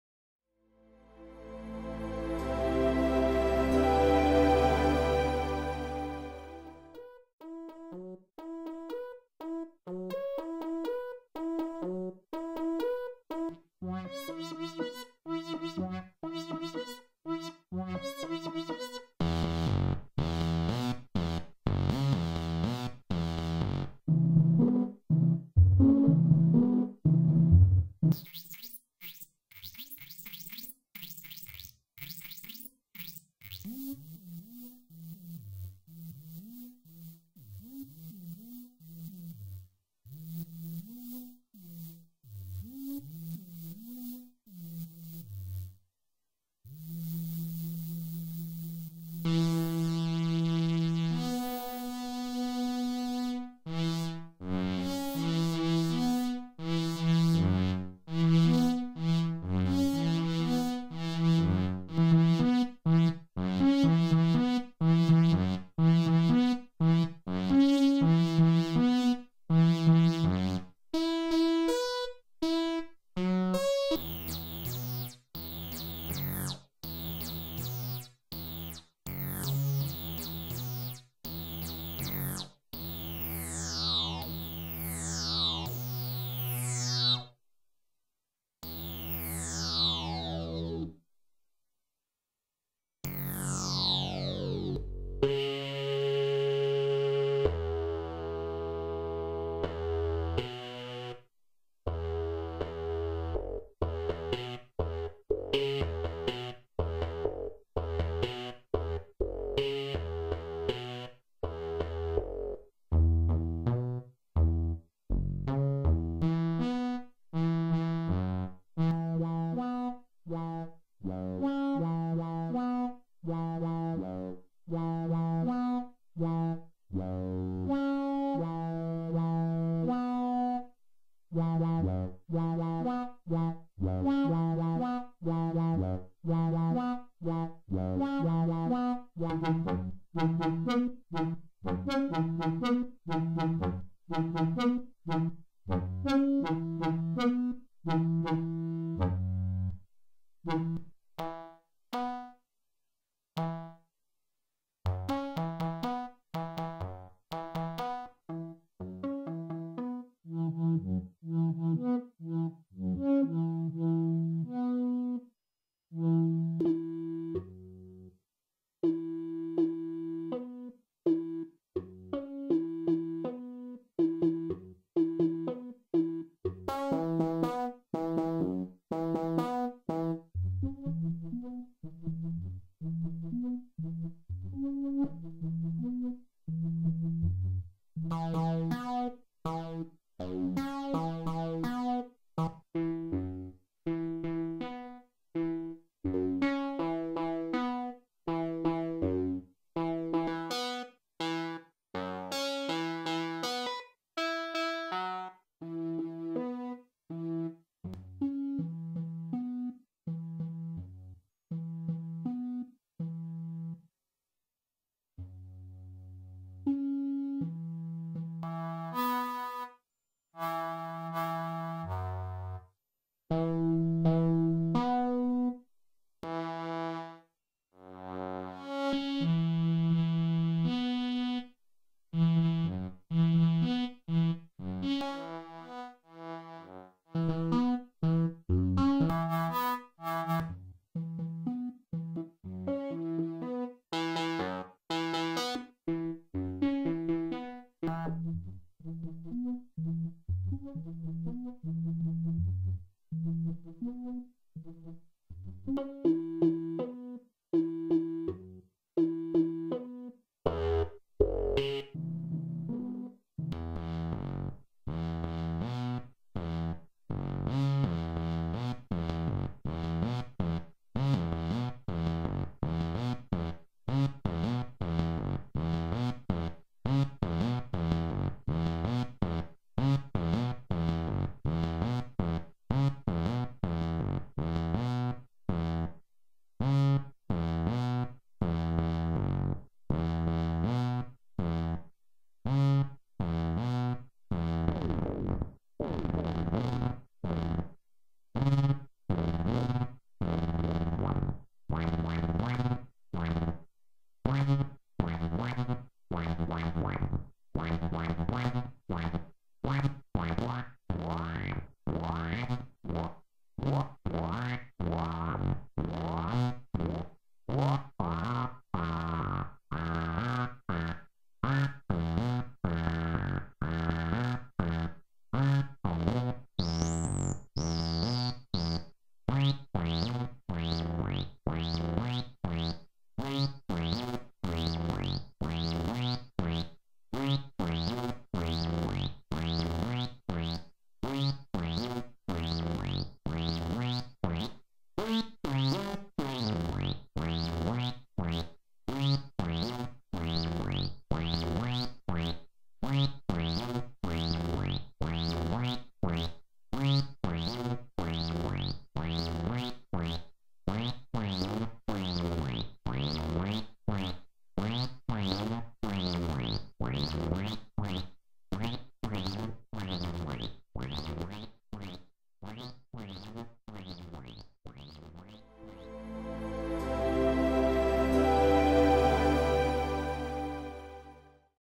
CS-80R Rick Jelliffe's
CS-80R is designed to emulate the "juiciness, funkiness and felinity" of the Yamaha CS- series of polyphonic analog synths.

make your own synth tutorial.
Make Your Own Software Synth, Part 1
Rick Jelliffe January 26 2005